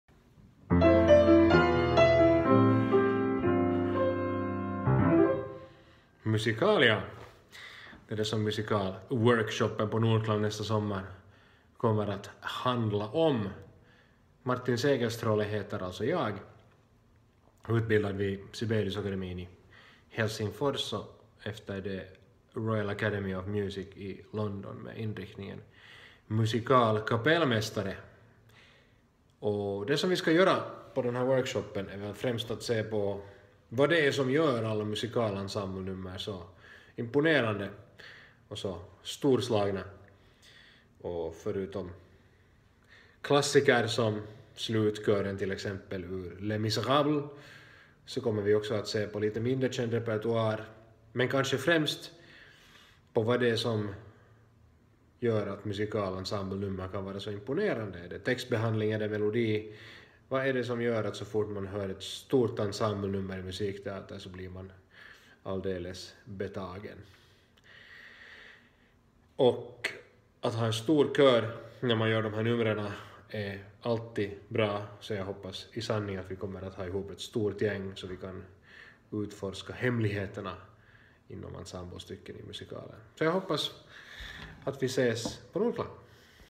Martin Segerstråle - Musikal
Martin Segerstråle är utbildad kapellmästare inom musikal vid Sibeliusakademin i Helsingfors och kommer att leda workshopen "Musikal" under Nordklang 2019. Här berättar han mer om vad deltagarna kan förvänta sig.

Martin Segerstråle is a graduate within musical direction from the Sibelius Academy in Helsinki and will be conducting the workshop "Musical" at Nordklang 2019.